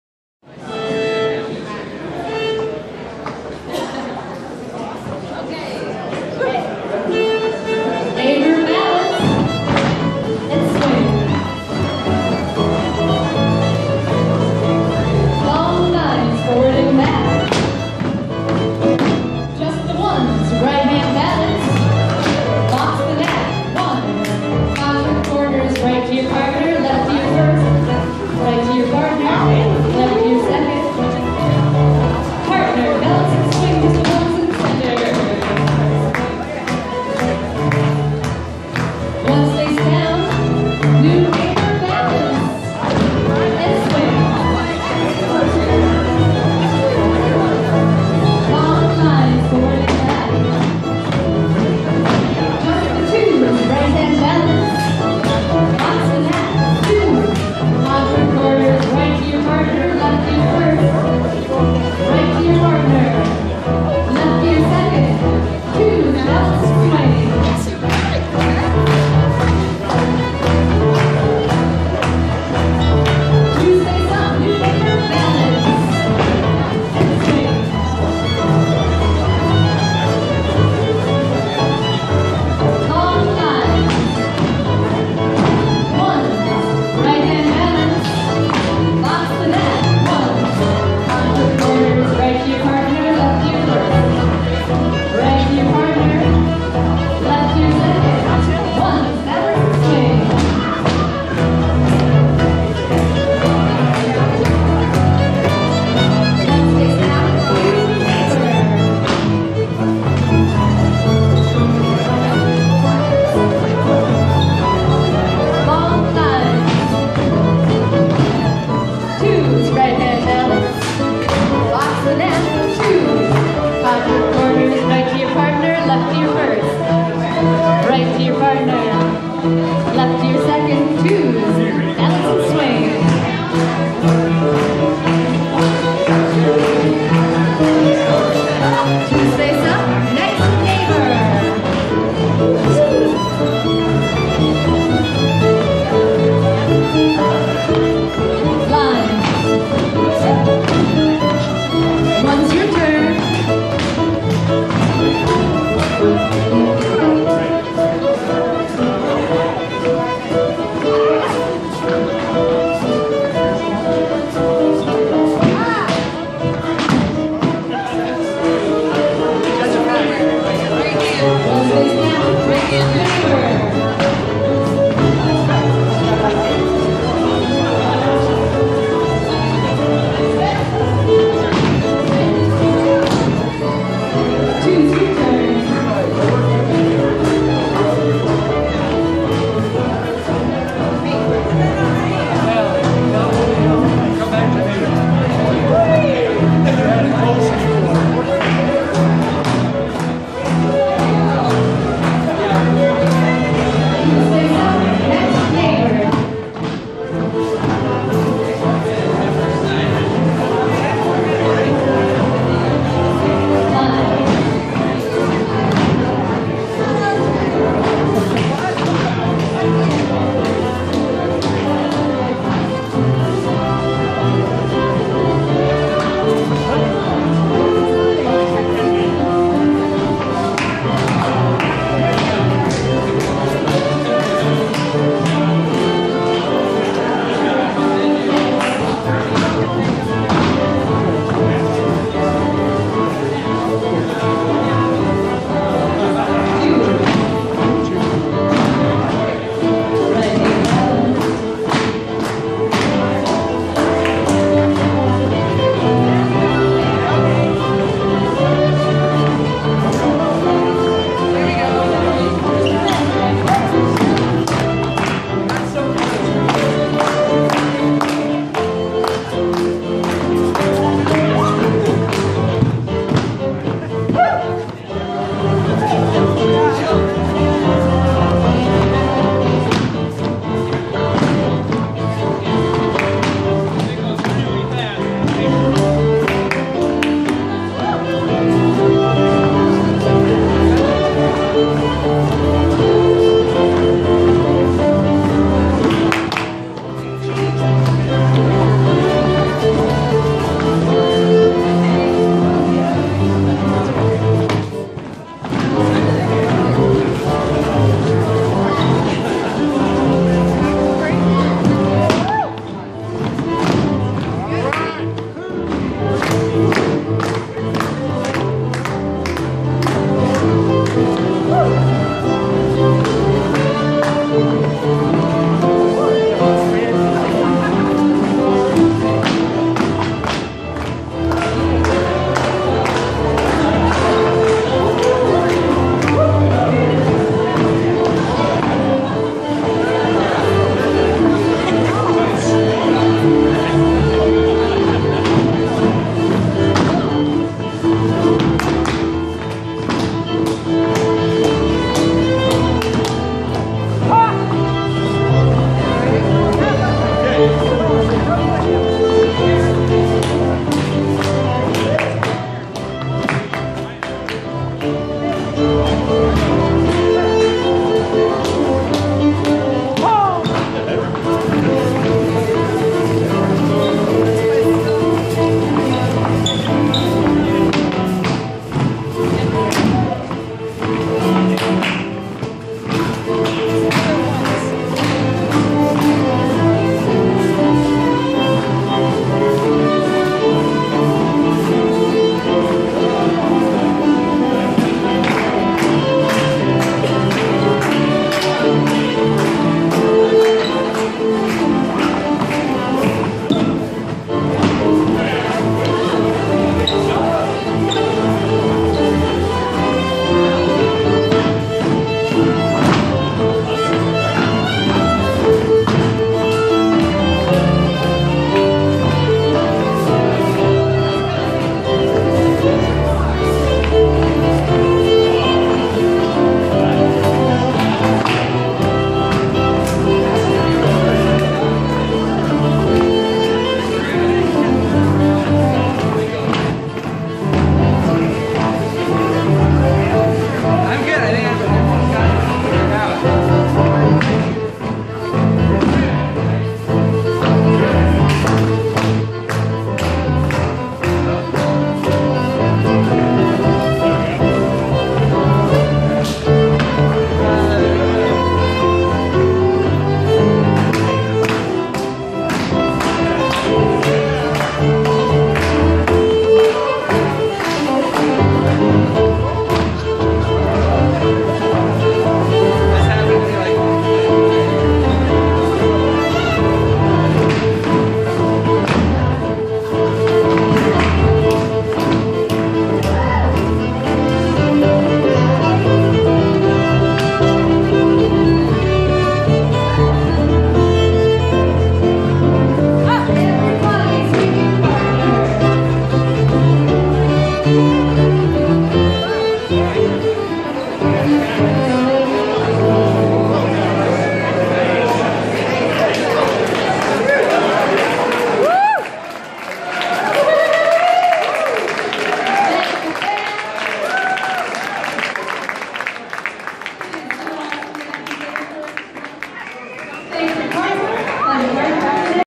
JosieScotty
Josie Toney and Scotty Leach played for the Denver Contra Dance on March 13, 2015. Christa Torrens called.